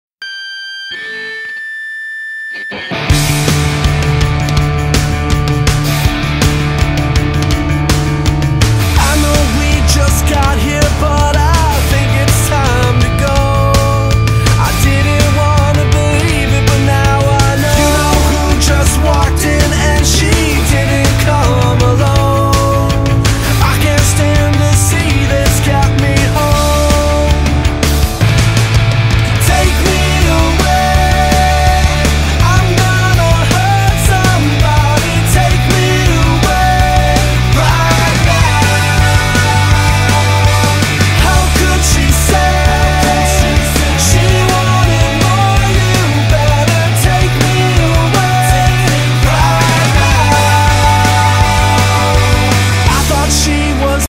taekwondo.black tip test. my son, joaquim .
here's joaquim, my son, aged 8 plus; doing his sparring for his black belt taekwondo test. his instructor, sir norman tan is in the background